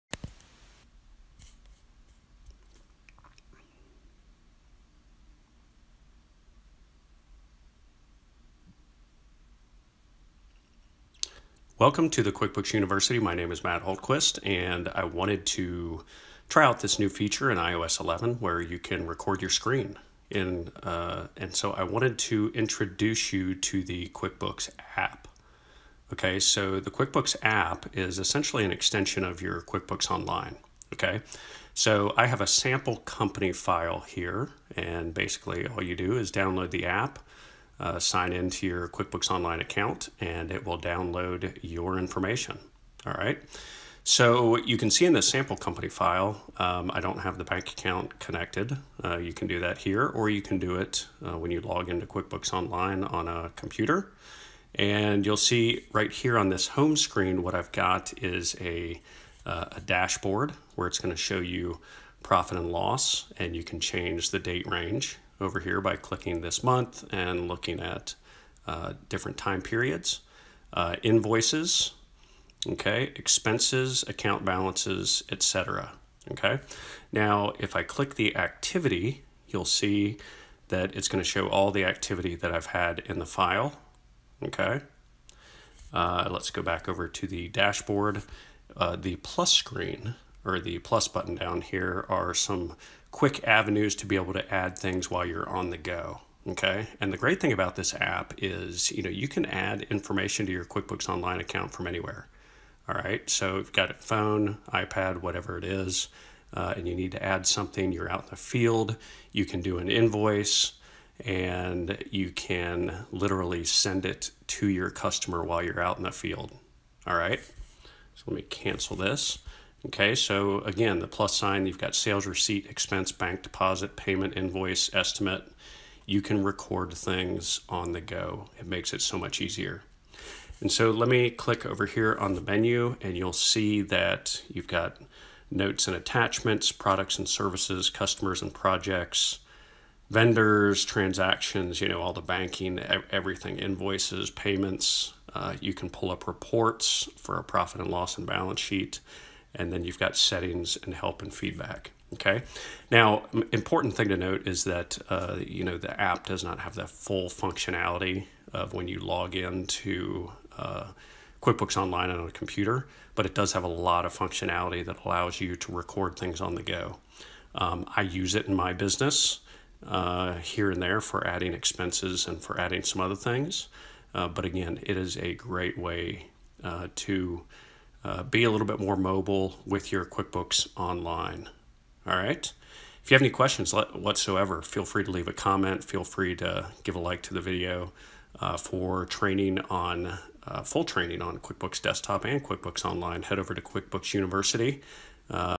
Quickbooks Online Tutorial - Overview of the Quickbooks App
- This video shows you a basic overview of the QuickBooks App available in the app store of your choice, and how to use it in your day-to-day business.  This is a great tool to use from anywhere when you are on the go!

For more free videos, full training series plus personal support for all your QuickBooks questions, please